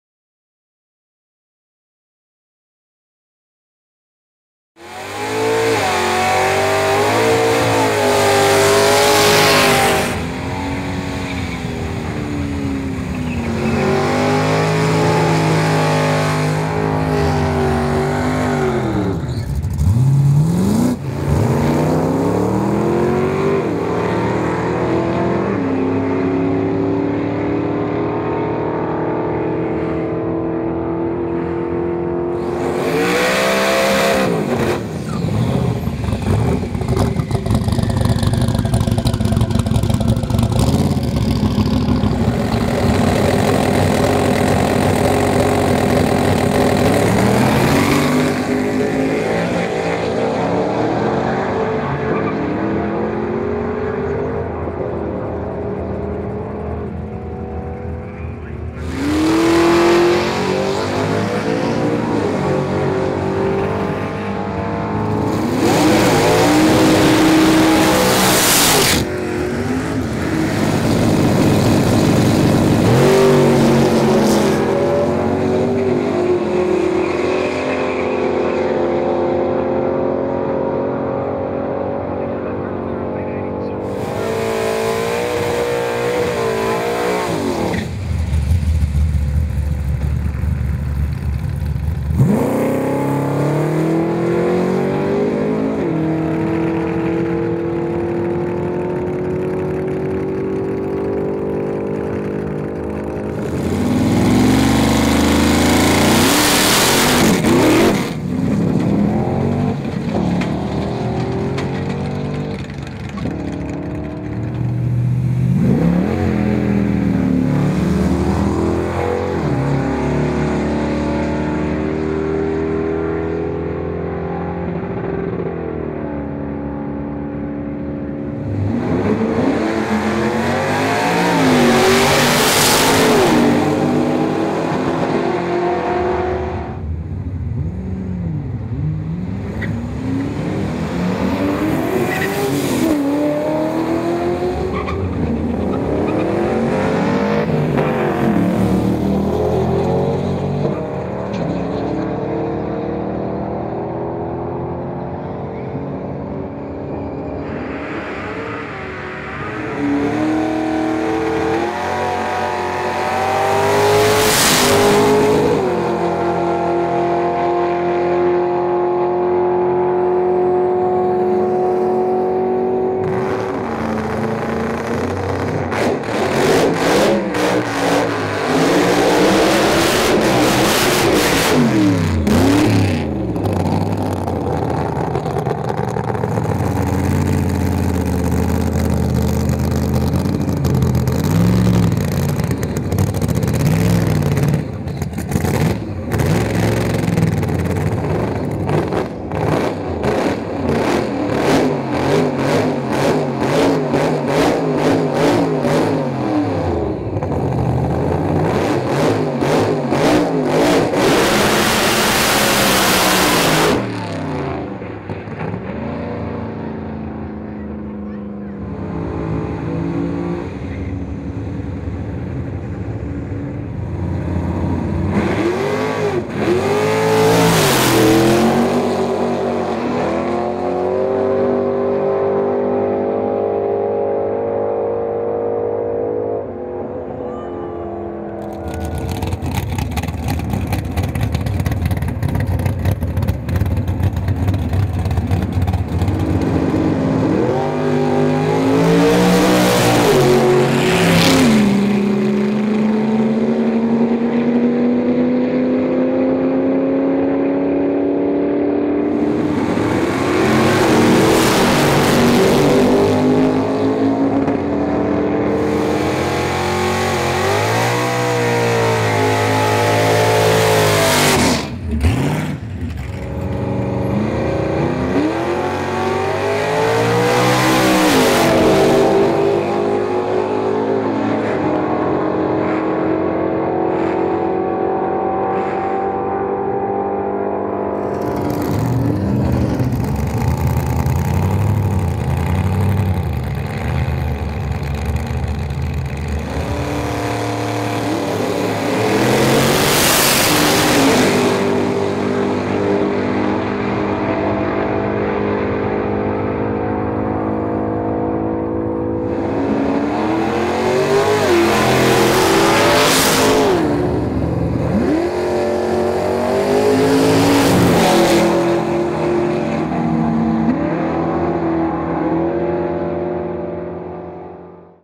Heathcote V8s - April 14th
Footage includes Ford Falcon, Holden Commodore, Torana, HSV V8 powered cars and dragsters plus more...
Heathcote Park Raceway, VIC, Australia - April 14th 2013